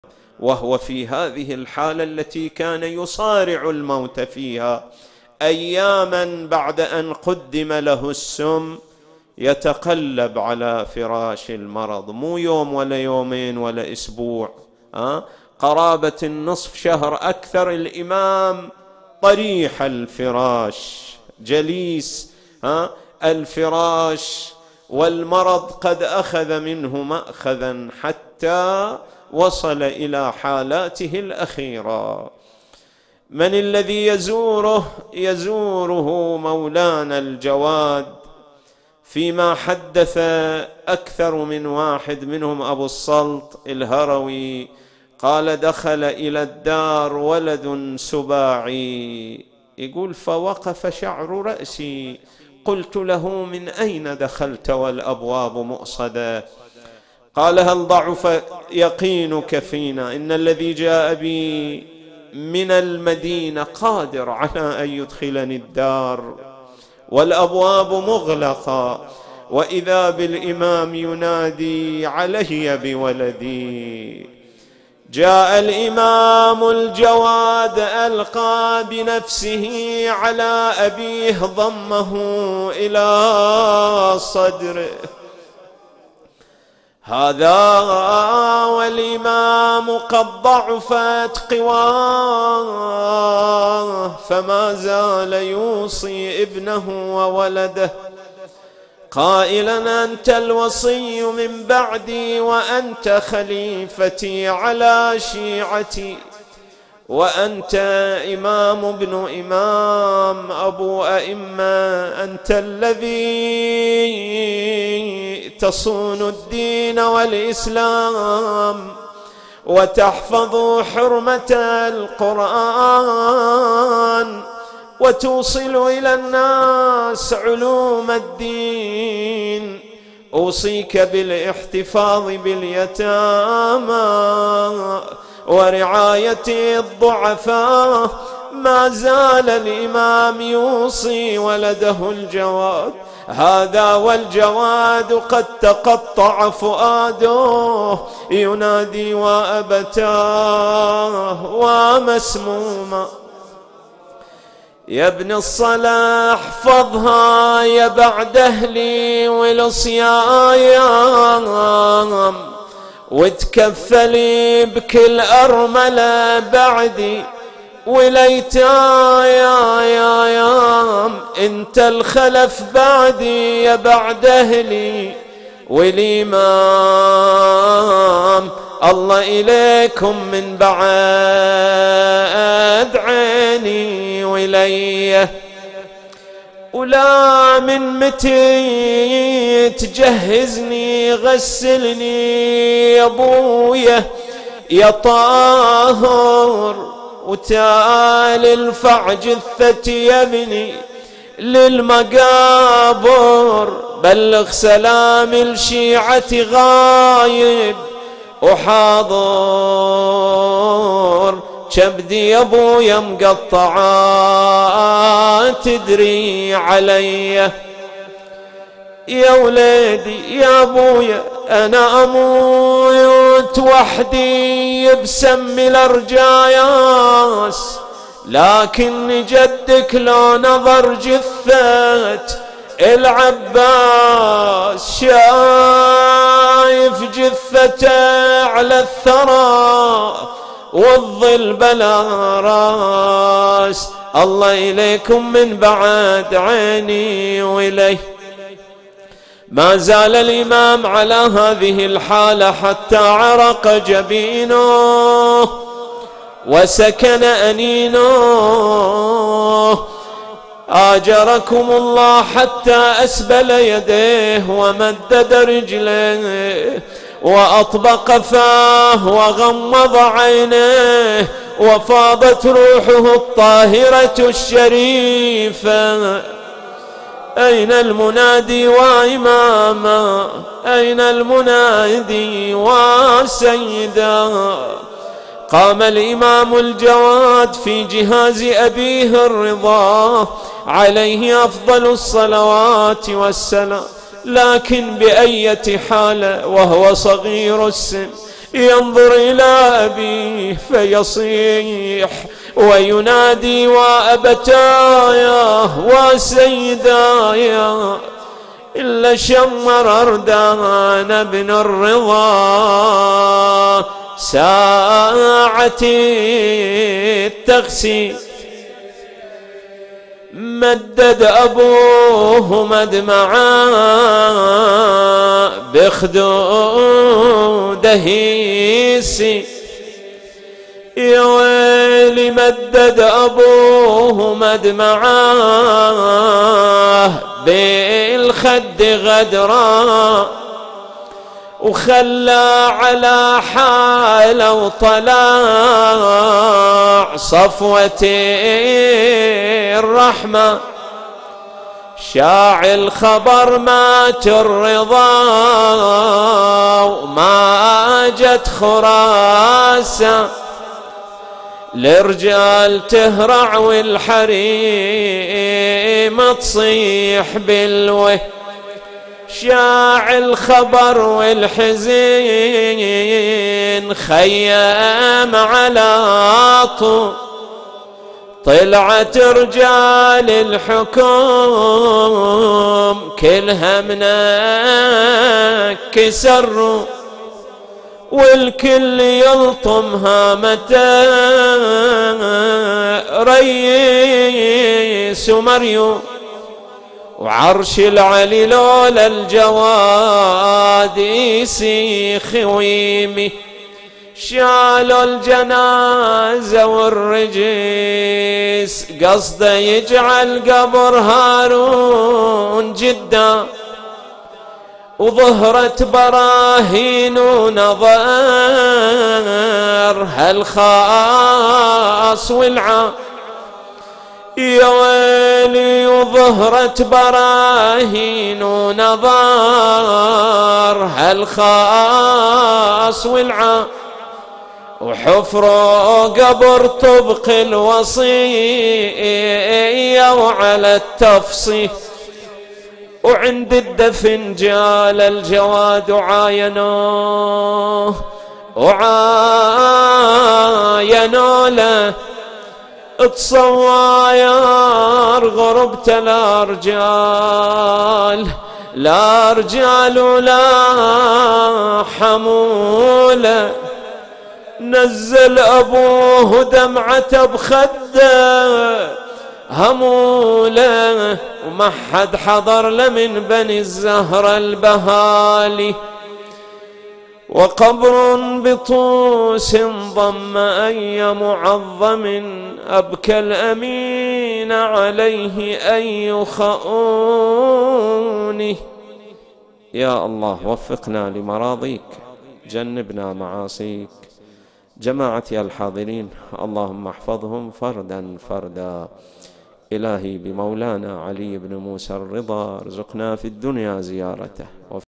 نعي استشهاد الإمام الرضا (ع) - الشيخ علي منصور سند - 17 صفر 1440هـ - مأتم المرخ
الشيخ علي منصور سند - استشهاد الإمام الرضا (ع) 17 صفر 1440هـ - مأتم المرخ